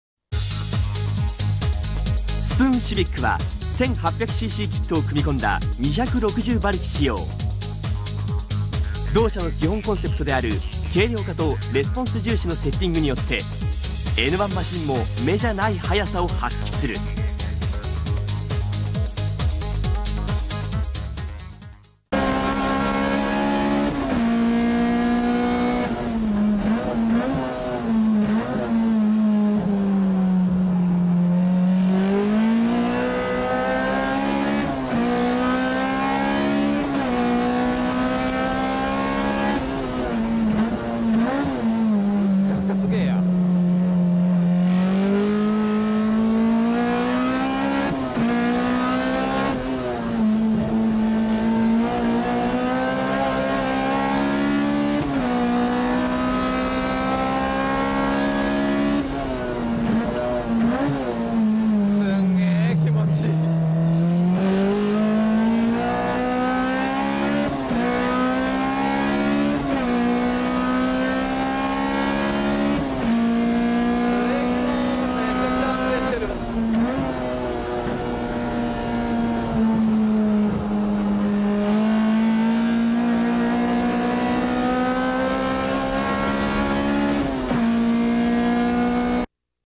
[ENG CC] Spoon Civic Type R EK9 260HP 900kg - full lap ONBOARD Tsukuba
Hondash - the ultimate tool to monitor your Honda
Keiichi Tsuchiya drives the fully built Spoon Civic Type R EK9 in the "N/A vs. Turbo" race.
This is the qualifying time attack lap.

Spoon Civic Type R Specs:
engine 1.8 kit 260HP:
-high cams
-lightened moving parts
-fully balanced engine
-Spoon prototype individual throttle bodies
-Spoon prototype Full Ti exhaust
-Spoon prototype ECU

Suspension:
- Showa based original suspension kit F16kg R14kg
- 4 pot calipers

Body:
- extreme weight reduction, cut out unnecessary metal panels without compromising structural integrity and rigidity
- 900kg (1040kg normal)

Transmission:
- Spoon close mission kit
- Final gear normal 4.4

Tires:
- Neova Advan A032RS 195/55/R15